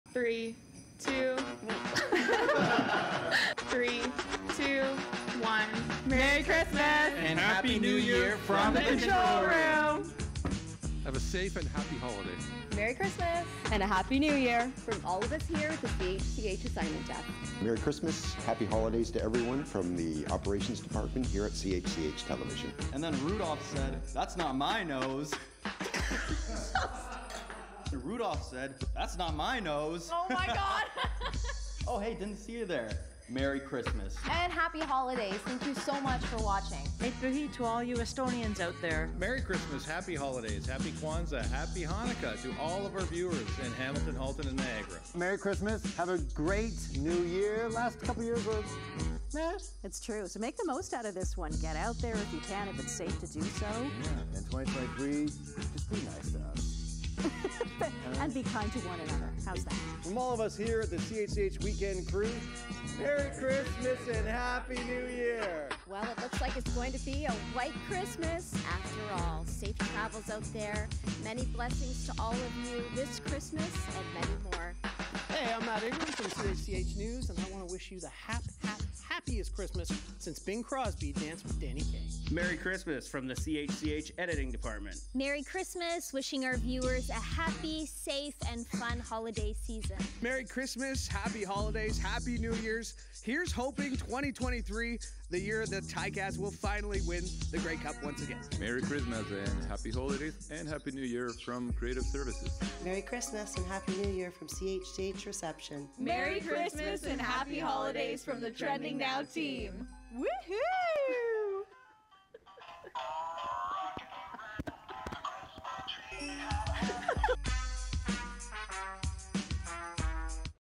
Happy Holidays from CHCH TV
The staff at CHCH TV want to wish our viewers a very Merry Christmas, Happy Holidays, and a Happy New Year.